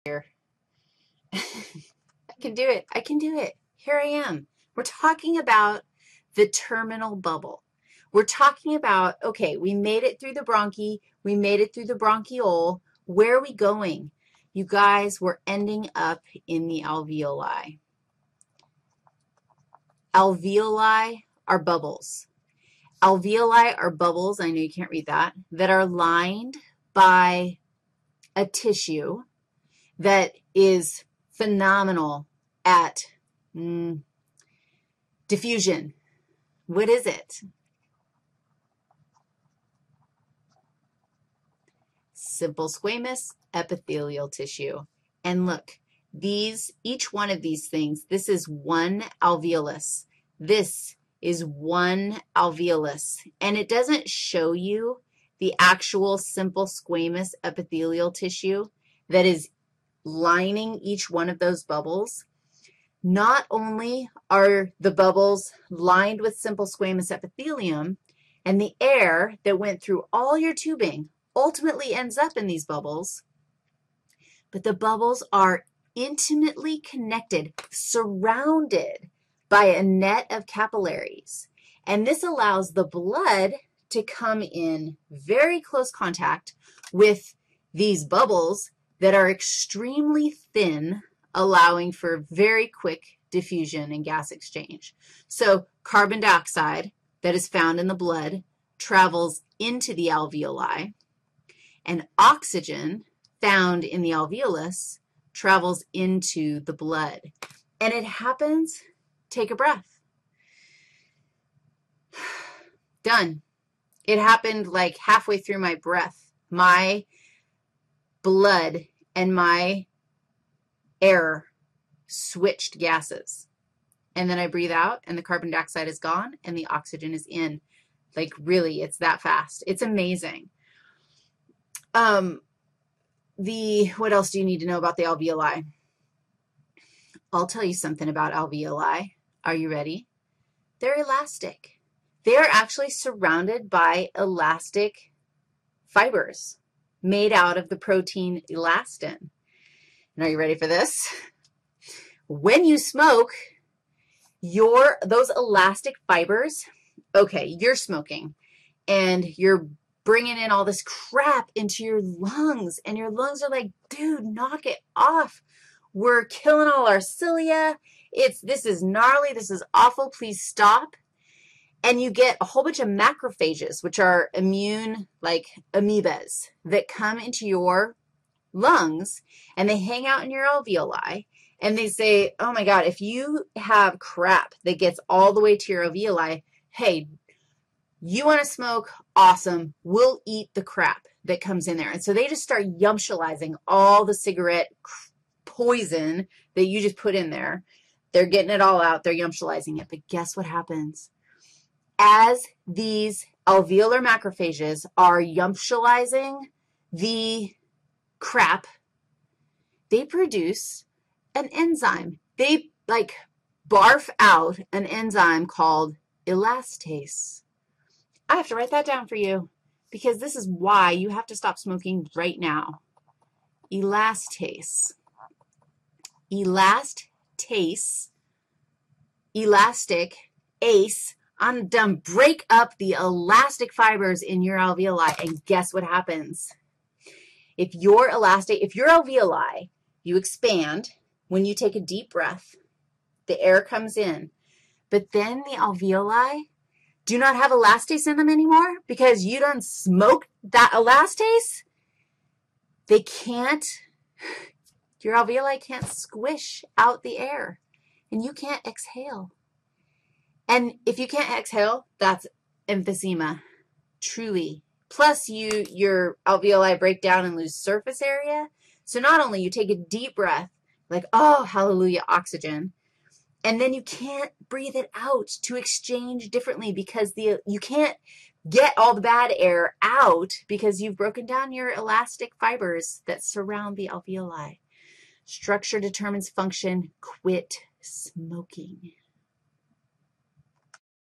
Respiratory system 8- Alveoli
Part 8 in an 10 part lecture on the respiratory system in a flipped Human Anatomy course taught by Wendy Riggs. CC-BY. Watch the whole lecture (all 10 videos) by going to the PLAYLIST